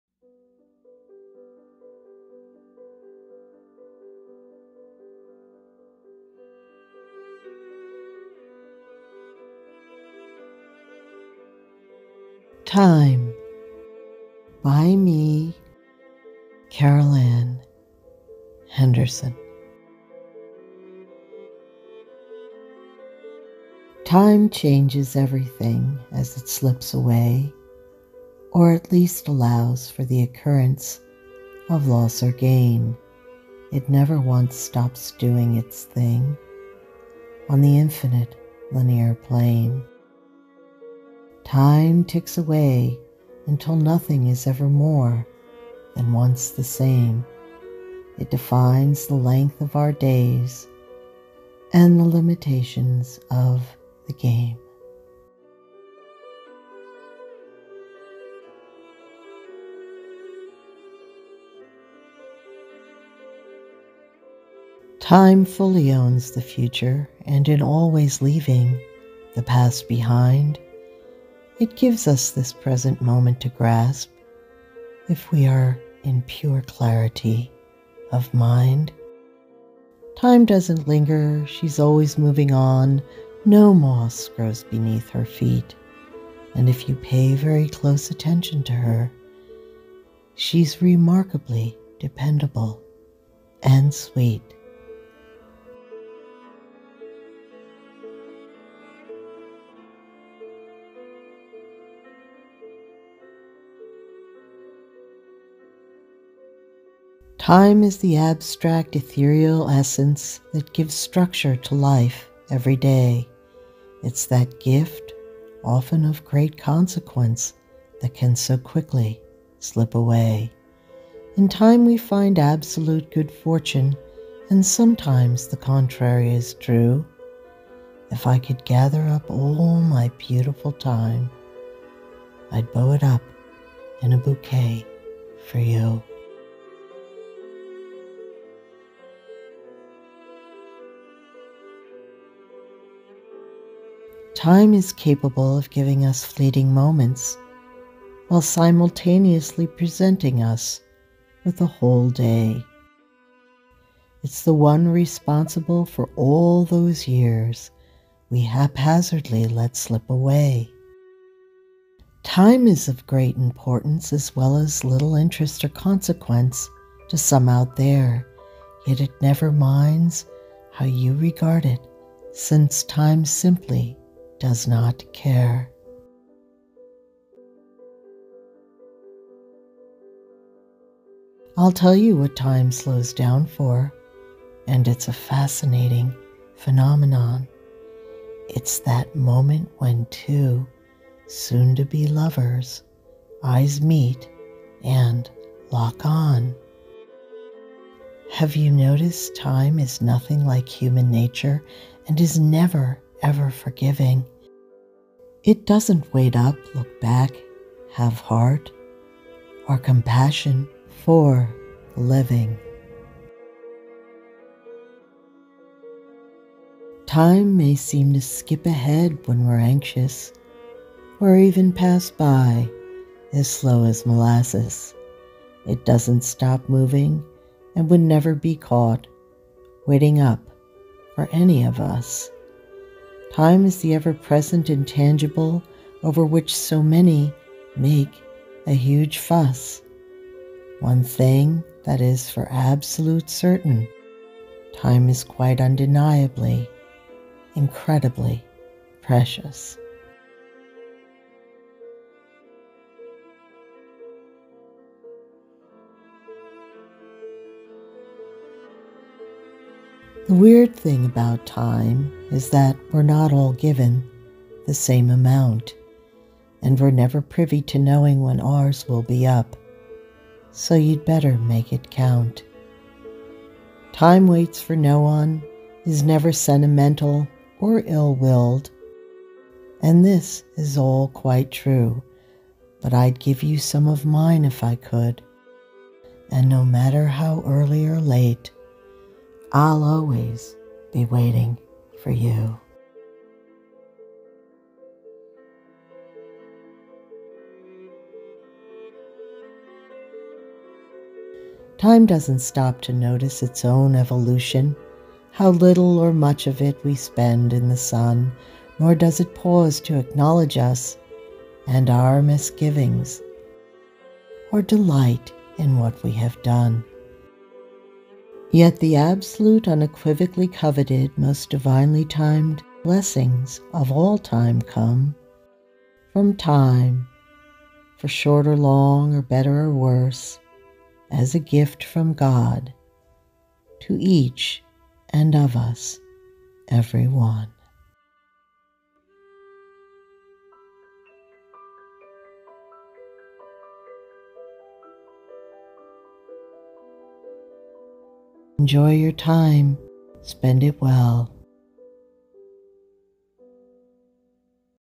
TIME ❤ A Love Story Within the Mystery of Something so Fleeting❤By Carol Ann Henderson
Greetings with Love from My Own Place in Time.

This video is a little notice of how precious our time actually is. It Isa gift from God to be cherished and made constructive good use of; be it in love, friendship, career, study, recteation, healing, and all manner the time in life we have been blessed with.

I hope you enjoy the video, and please serve yourself in remembering that we are each on our own individual path, as well as, and more importantly to be aware of, a different place on pur path towards freedom, enlightenment, self awareness, discovery, relationships and anything else you may be moving towards.

I love you. God loves you. And time is but a fickle friend.

Love Always, Firever, and Through the Akashic,
Carol Ann

❤Make It A Good Time In Love❤